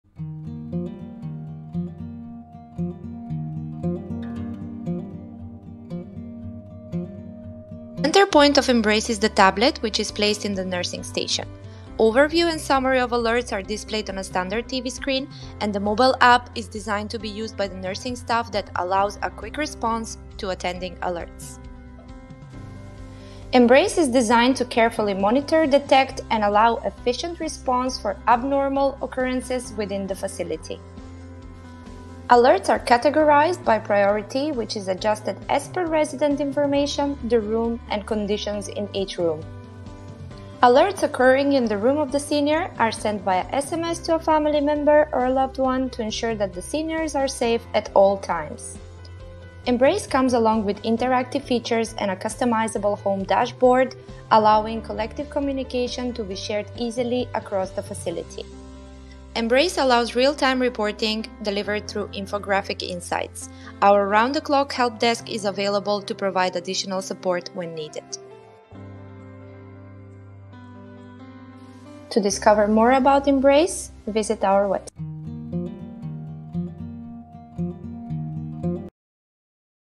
PLA Embrace
We are please to introduce Embrace, an alert system that will save you money!  Embrace allows you to monitor all of the rooms in your facility at one time.

No cameras, no pendants, no wrist bands!!!

Embrace detects falls, changes in behavior and much more.

Embrace is easy to use, install and almost free.  It is a  customizable system built for facilities.